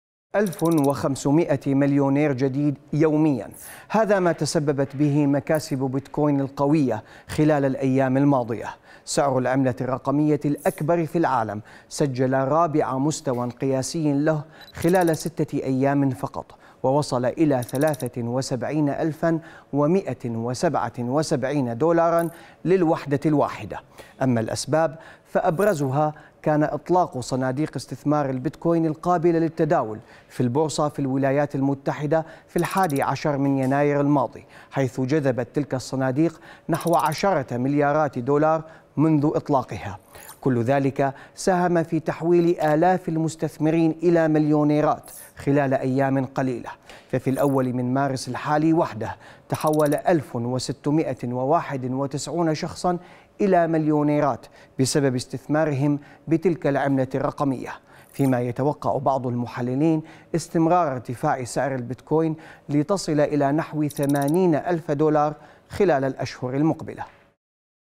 بفضل ارتفاعات "بيتكوين".. 1500 مليونير جديد يوميا
سجلت رابع مستوى قياسي لها خلال 6 أيام فقط.. 1500 مليونير جديد يوميا بفضل ارتفاعات "بيتكوين"
لمعرفة أحدث المستجدات.. عبر البث المباشر من هنا ⤵
وبإمكانكم متابعة العربية على منصات التواصل